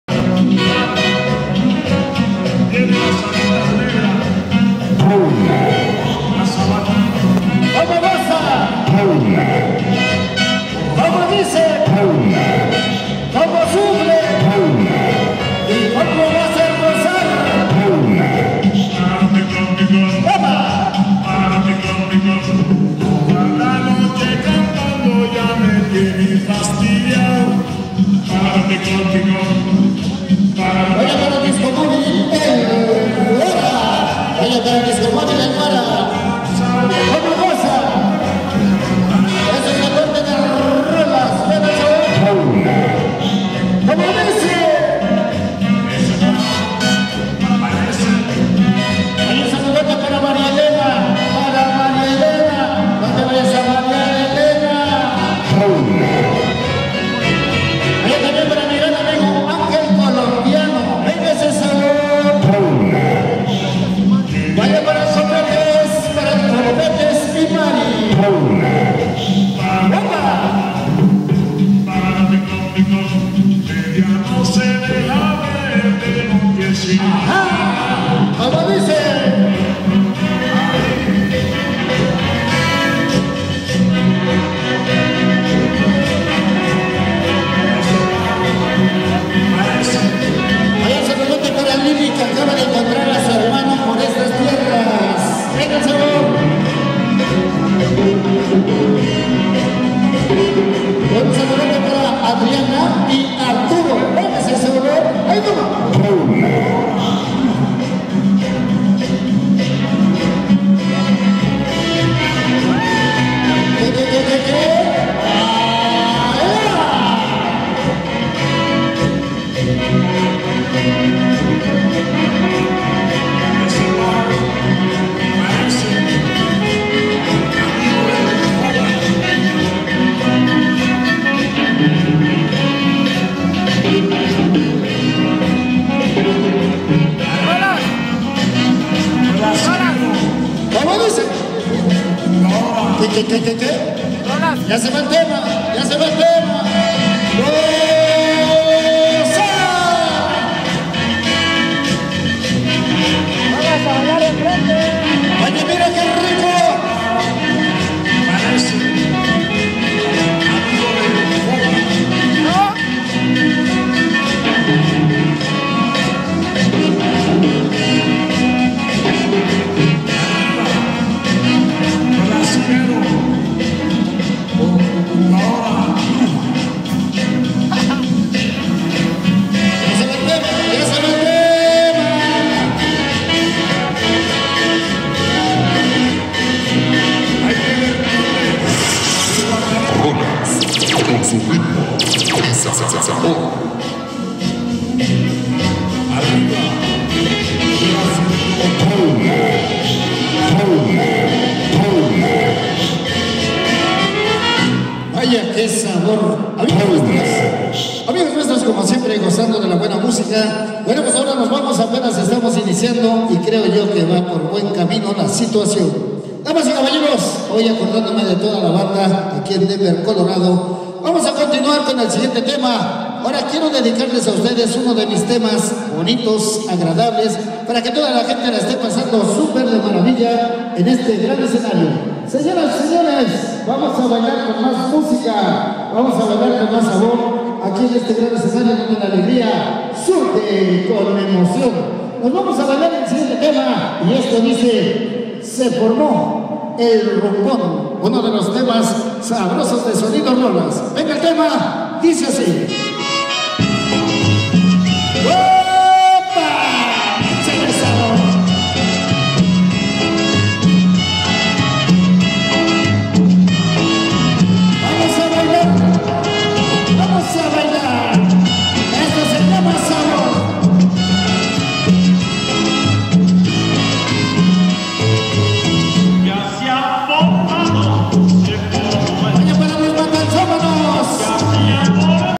Sonido rolas en Denver colorado
Un ídolo del ambiente sonidera uno grande el cual compartimos ecenario con Rolando en rabioso sonido sensación rubí y sonido versátil pues nos llevamos un buen ambiente y una persona maravillosa la cual empa ganando un amigo más saludos mi Rolando